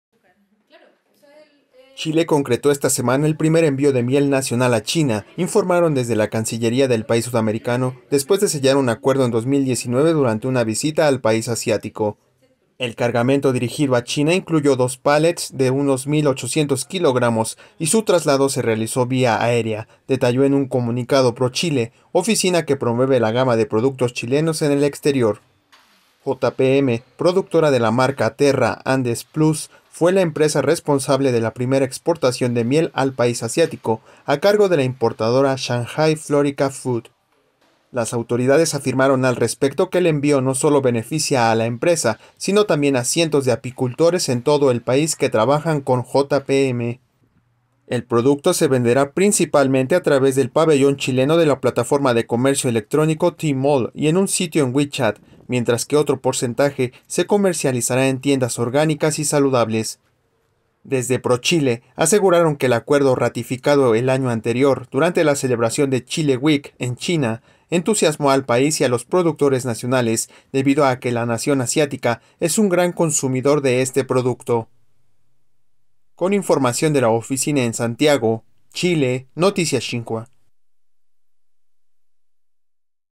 Chile concreta primer envío de miel nacional a China
Chile concretó esta semana el primer envío de miel nacional a China, informaron desde la cancillería del país sudamericano, después de sellar un acuerdo en 2019 durante una visita al país asiático.